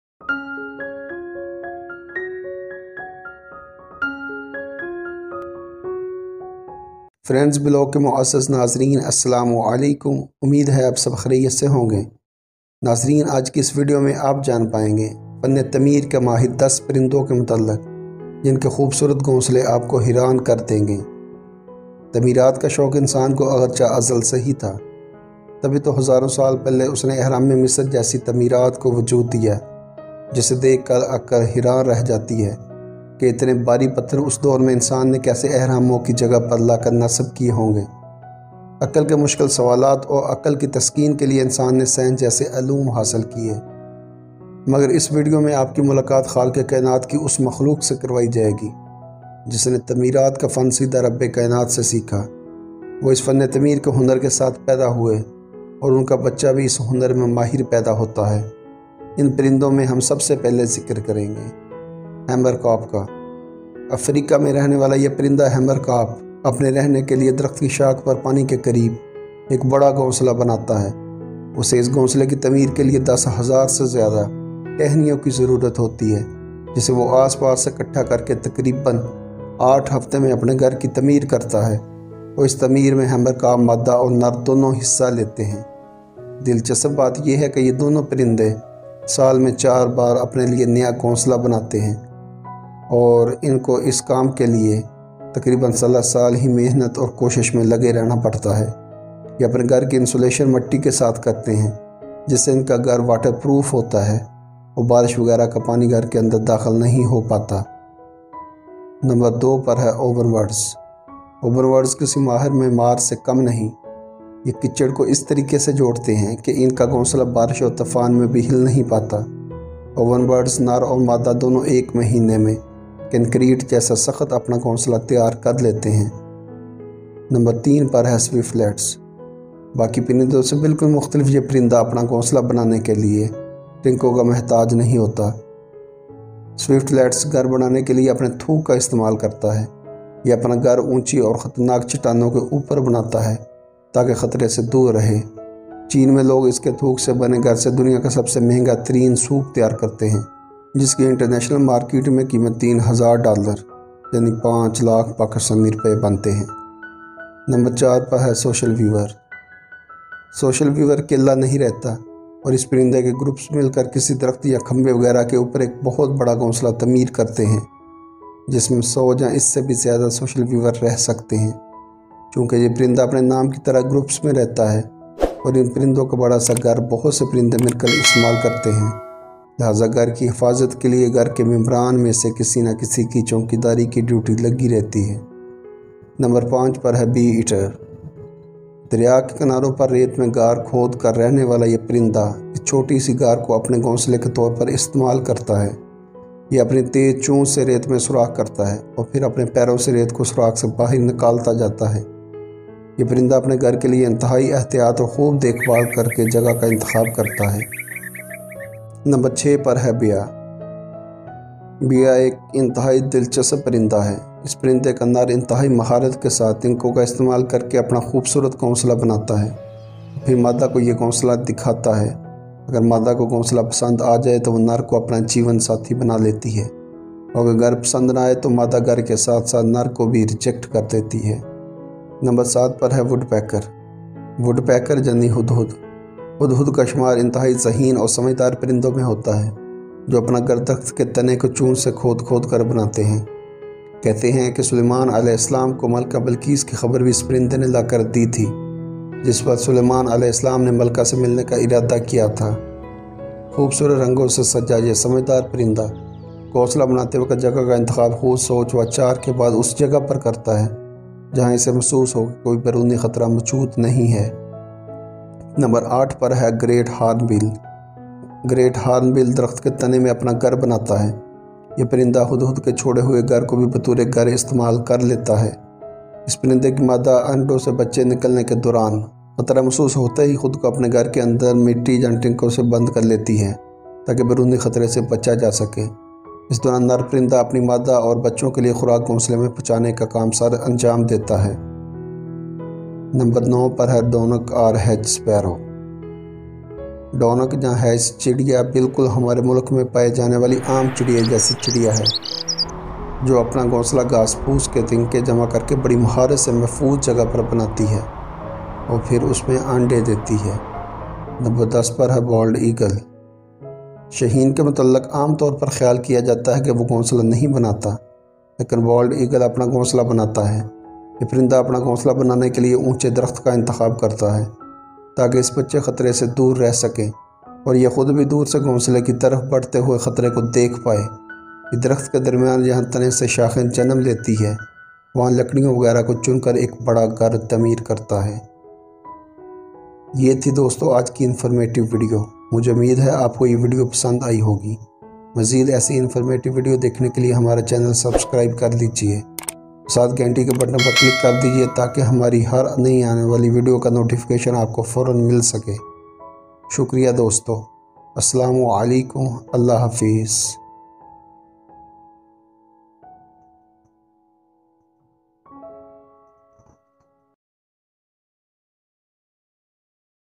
10 Most Amazing Nests In The Animals World! | Urdu/Hindi
In this video, you will be know about most amazing, beautiful and interesting nests of these birds
Hammerkop
Ovenbirds
Swiftlets
social weaver
Bee-eater
Baya
Woodpecker
Great Hornbill
Dunnock or Hedge Sparrow
Bald eagle

Shahzad info tv